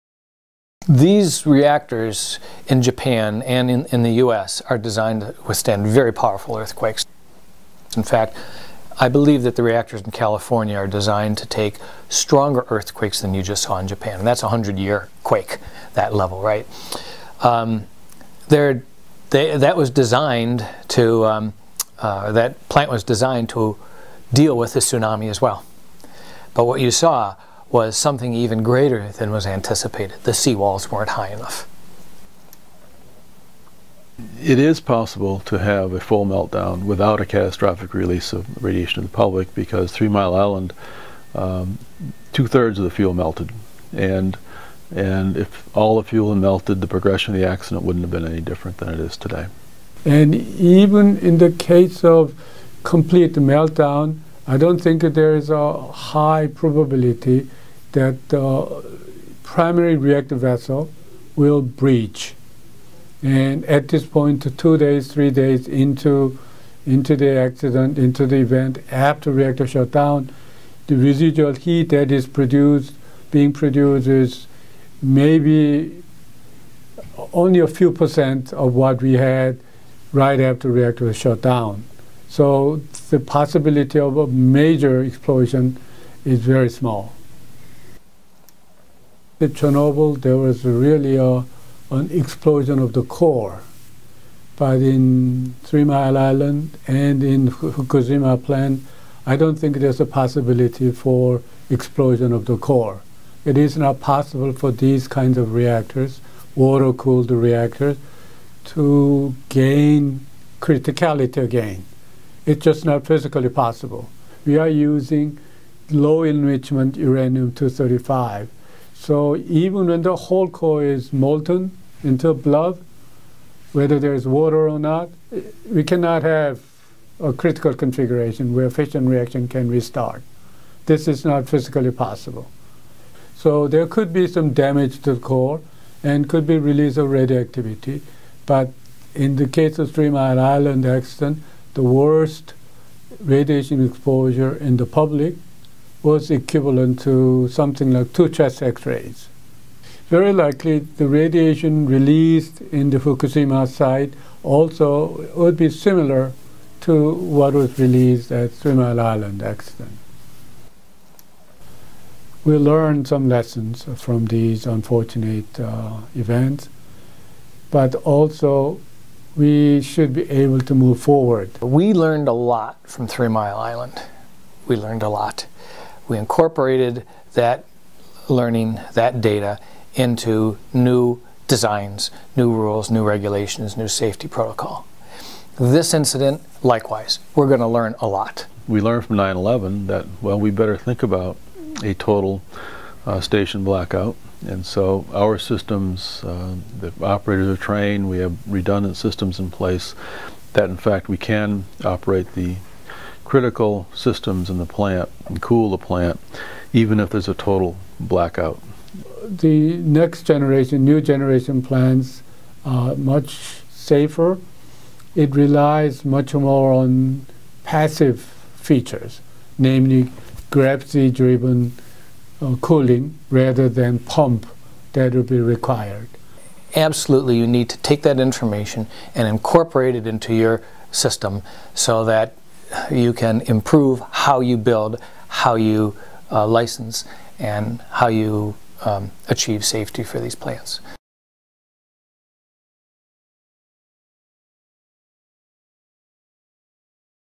Japan Nuclear Emergency: Experts from Univ. of Michigan Discuss Critical Issues
Experts from the department of Nuclear Engineering and Radiological Sciences at Michigan Engineering discuss some of the critical issues surrounding the nuclear emergency in Japan. Questions and issues addressed: Does "meltdown" mean a catastrophic radiation release? Why a Chernobyl scenario is impossible. This video was shot on the morning of March 14, 2011.